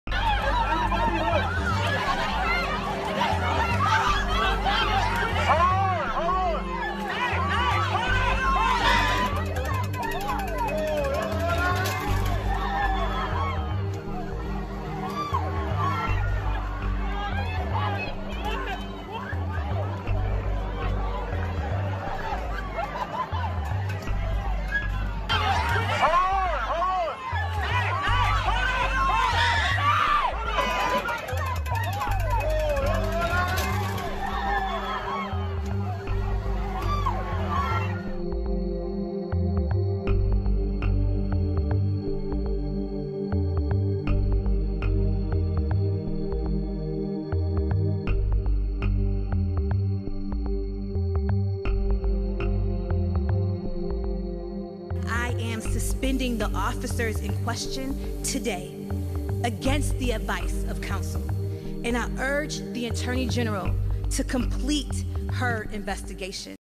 Caught on cam: Vehicle drives into crowd during NYC protest
A car plowed through protesters in Times Square during a demonstration following the release of footage of the arrest of Daniel Prude.